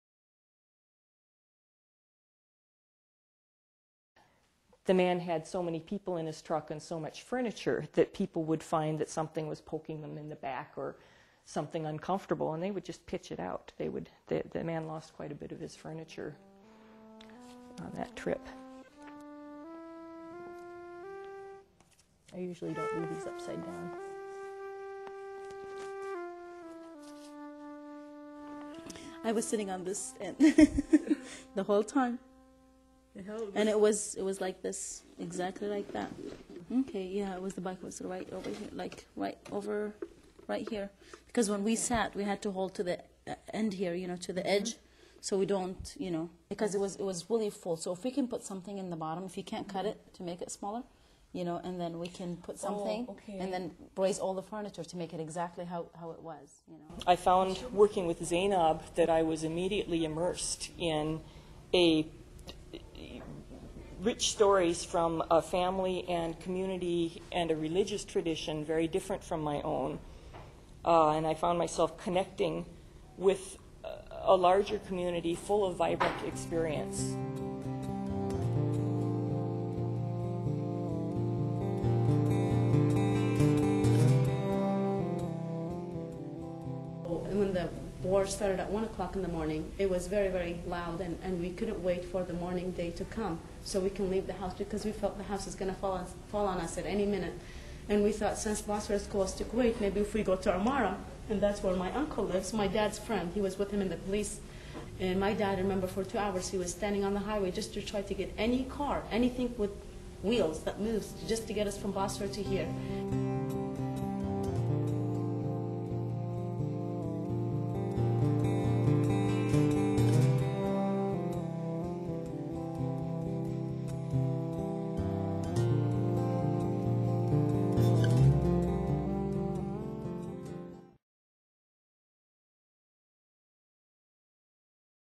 Kate Brooke - Stories of Home
Lincoln Arts Council's "Stories of Home" public art project documentary, with artist Kate Brooke.